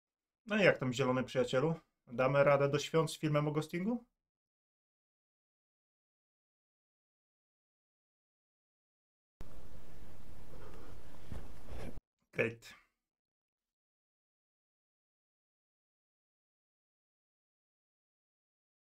Ghosting dla zielonych - zapowiedź
Co prawda dzisiaj, pomimo że jest piątek, specjalnego filmu nie będzie, bo zgodnie z zapowiedziami publikuję mecze turniejowe, ale film powstaje. Dzisiaj wrzucę chociaż jego zapowiedź, może kogoś zachęci, może pobudzi ciekawość (trochę na to liczę). Na pewno dla mnie to fajna odskocznia od meczy, bo to bardzo monotonne zajęcie i czasem trzeba się oderwać.

A jeśli podoba Ci się to co robię i chcesz żeby tego było więcej i lepszej jakości, możesz wesprzeć moją działalność przez